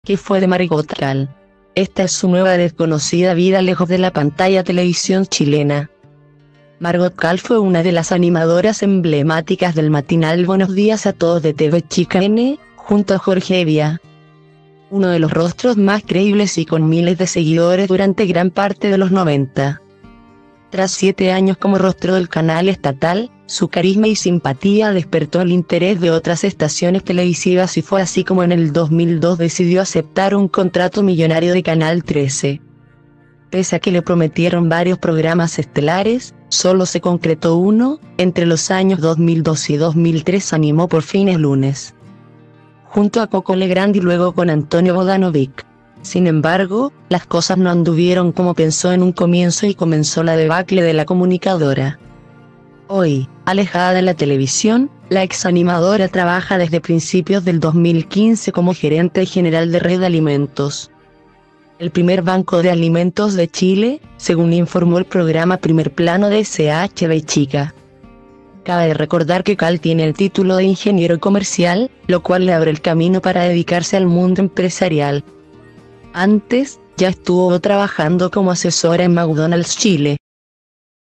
¿Qué fue de Margot Kahl? Esta es su nueva desconocida vida lejos de la pantalla | TV chilena
Margot Kahl fue una de las animadoras emblemáticas del matinal Buenos Días a Todos de TVN – junto a Jorge Hevia-,uno de los rostros más creíbles y con miles de seguidores durante gran parte de los ’90. Tras siete años como rostro del canal estatal, su carisma y simpatía despertó el interés de otras estaciones televisivas y fue así como en el 2002 decidió aceptar un contrato millonario de Canal 13.